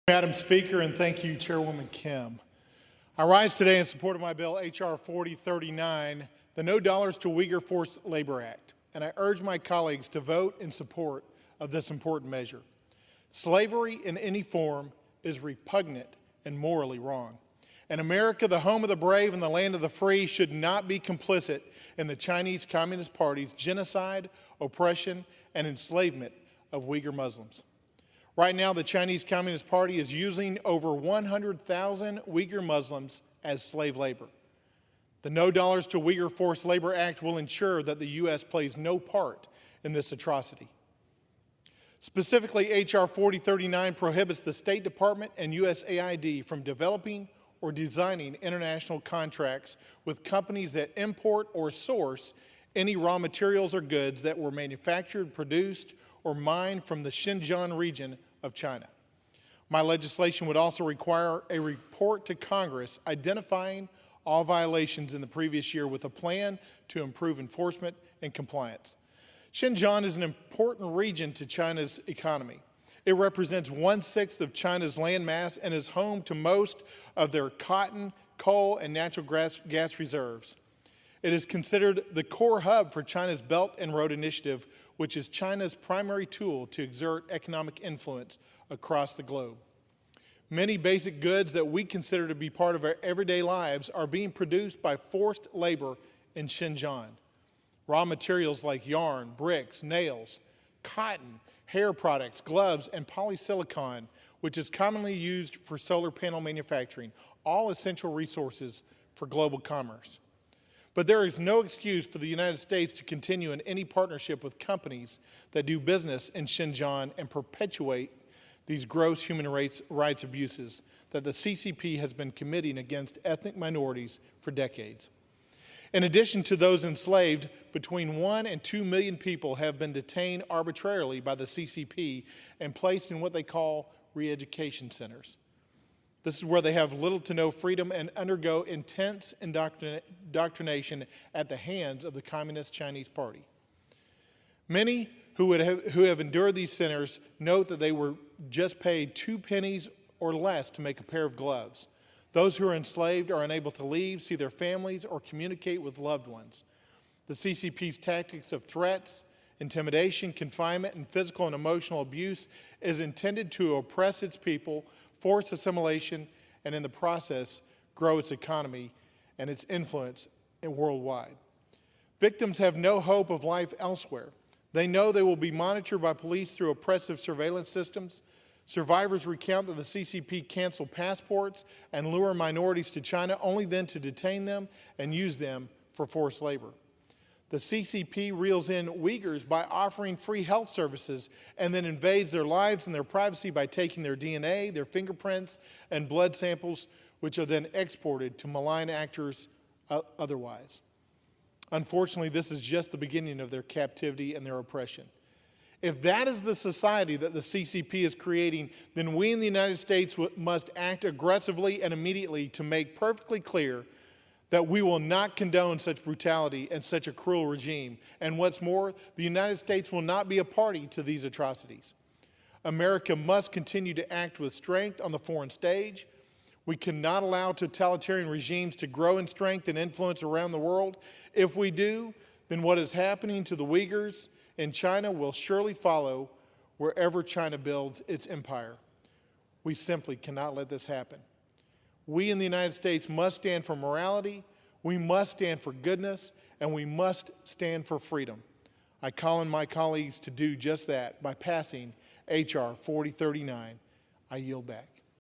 U.S. House passes Rep. Moran’s No Dollars to Uyghur Forced Labor Act
Congressman Nathaniel Moran (TX-01) delivers remarks on his legislation, H.R. 4039, No Dollars to Uyghur Forced Labor Act.